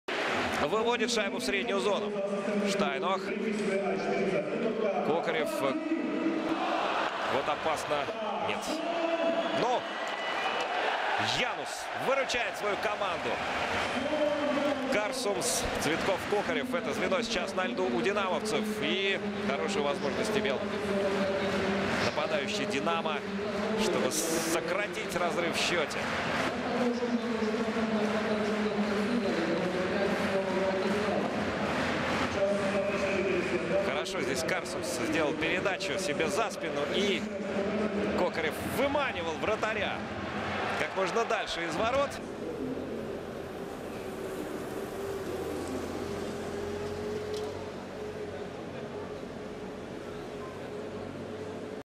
Ярослав Янус на чеку / Jaroslav Janus makes nice pad save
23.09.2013, Ярослав Янус успевает сделать движение щитком и преградить путь шайбе, пущенной в ворота